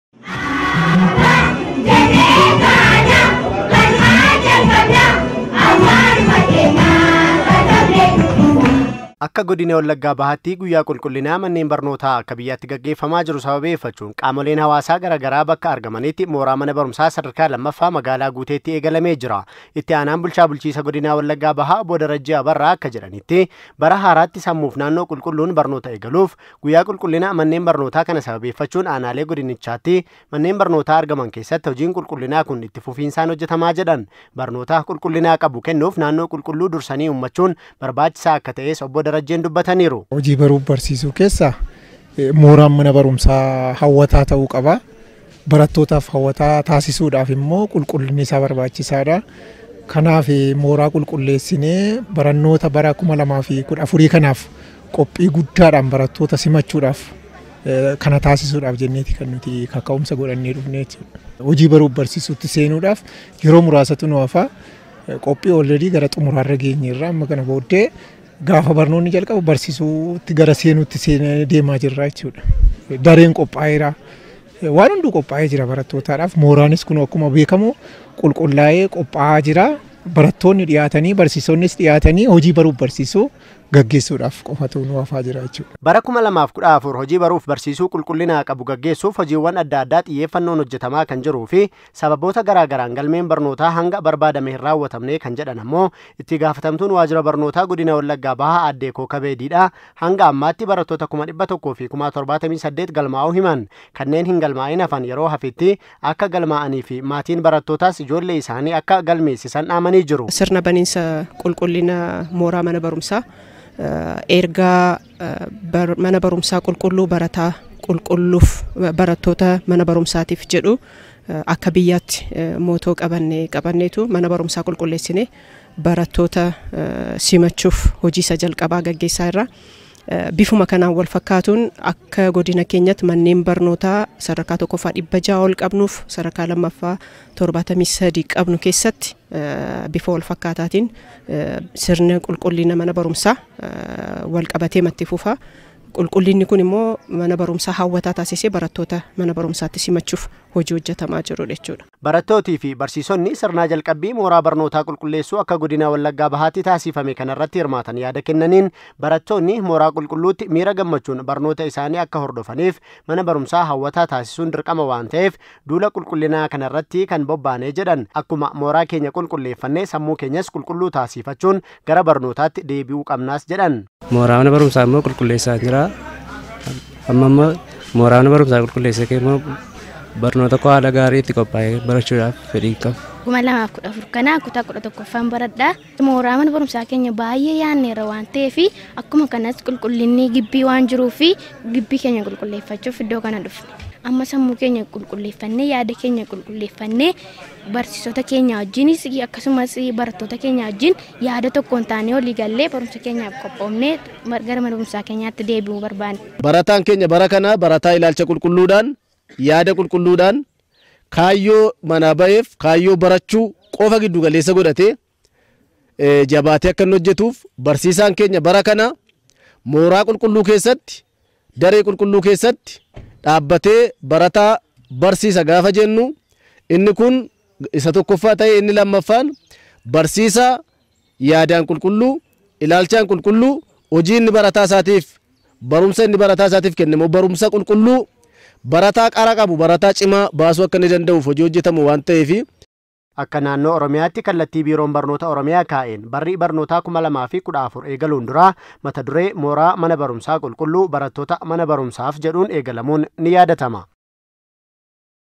Addis Miidiyaa Neetwoorki (AMN) Fulbaana 11/2014
Manneen barnootaa hawwatoo taasisuun hojii baruu fi barsiisuu qulqullina qabu kennuuf hunduu tumsuu akka qabu, Waajjirri Barnootaa Godina Wallagaa Bahaa Beeksise.
Guyyaa Qulqullina Manneen Barnootaa Sababeeffachuun Mana Barumsaa Sadarkaa 2ffaa Magaalaa Guuteetti Hojiin mana barnootaa Qulqulleessuu gaggeeffameera.